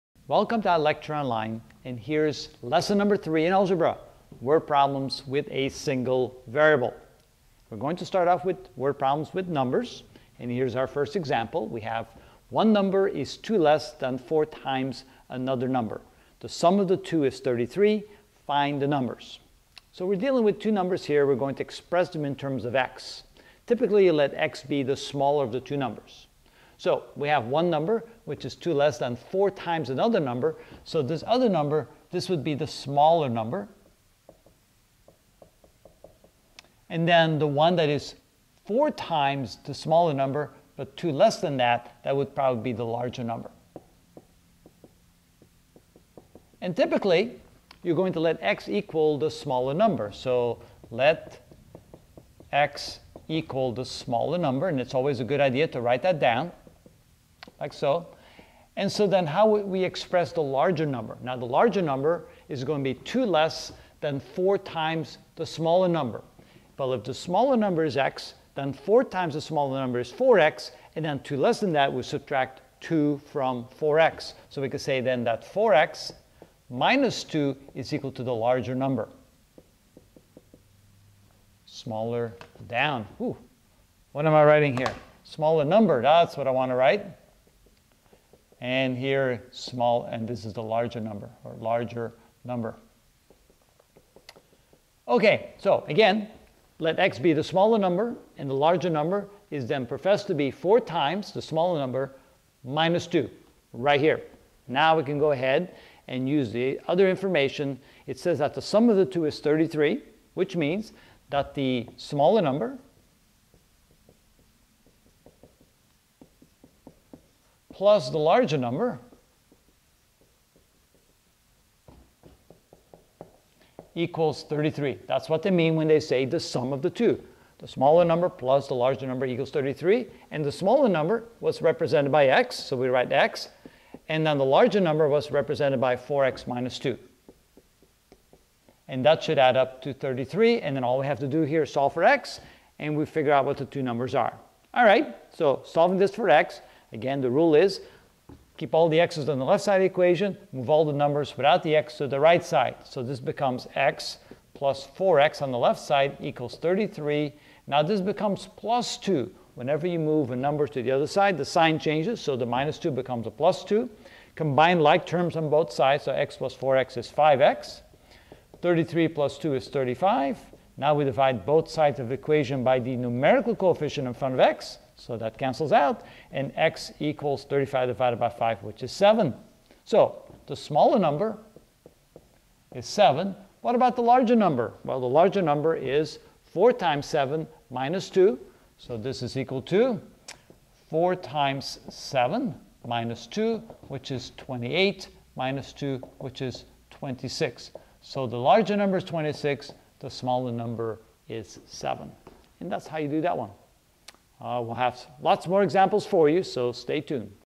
Algebra - Word Problems: Numbers (1 of 2)
Word problems are challenging, but rewarding once you learn how they're done!  Let's take a look at another word problem, this time we'll try a problem that deals with more generic numbers.

Problem Text: One number is 2 less than 4 times the other.  The sum of the two is 33.  Find the numbers.